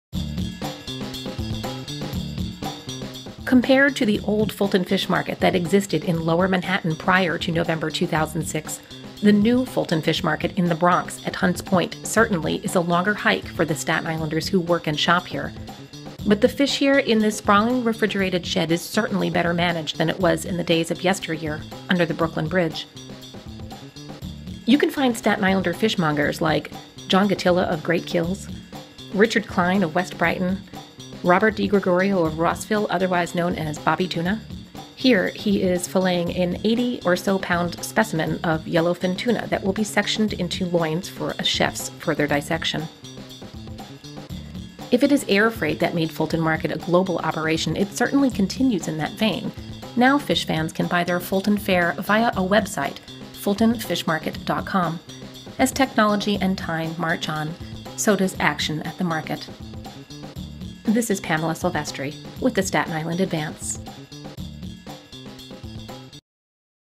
Fulton Fish Market At Hunts Point
The market continues its seafood traditions in the Bronx